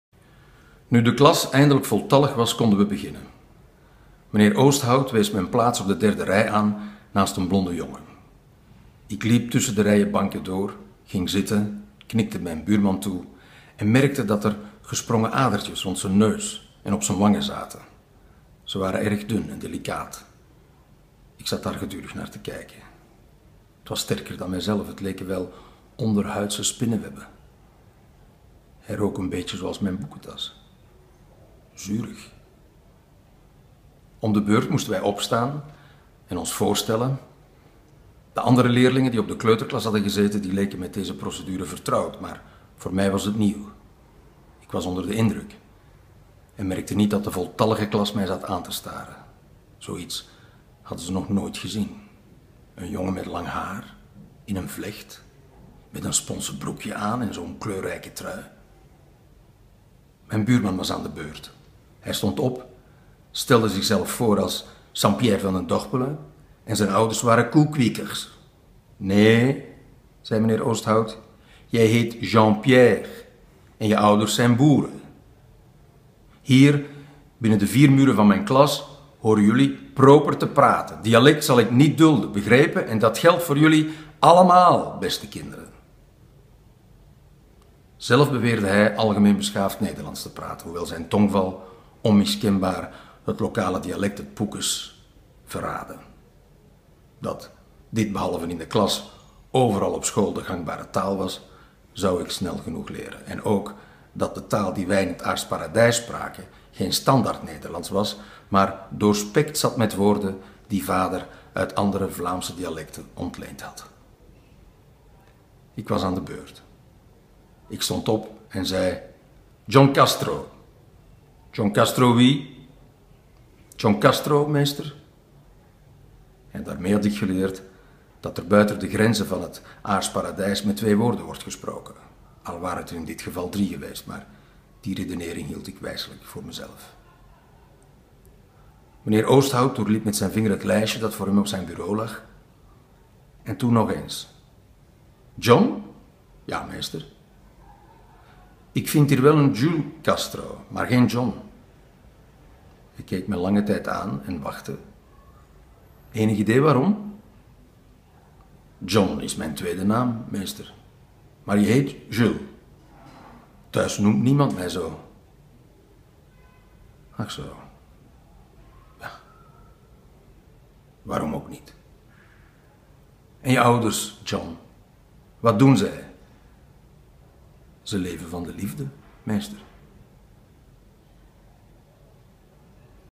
Acteur Dirk Roofthooft leest voor uit 'Aards Paradijs', het debuut van Seppe van Groeningen.
Dit filmpje is gebruikt voor de boekvoorstelling van 'Aards Paradijs' in Concertzaal de Vooruit Gent op 16 oktober 2014.